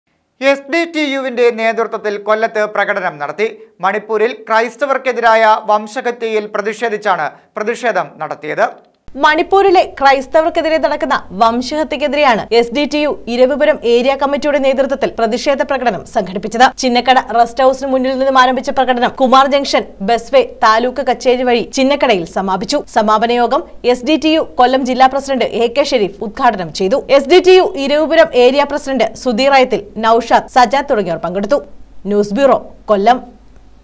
എസ്.ഡി.റ്റി.യുവിന്റെ നേതൃത്വത്തില്‍ കൊല്ലത്ത് പ്രകടനം നടത്തി
എസ്.ഡി.റ്റി.യു വിന്റെ നേതൃത്വത്തില്‍ കൊല്ലത്ത് പ്രകടനം നടത്തി. മണിപ്പൂരില്‍ ക്രൈസ്തവര്‍ക്കെതിരായ വംശഹത്യയില്‍ പ്രതിഷേധിച്ചാണ് പ്രതിഷേധം നടത്തിയത്.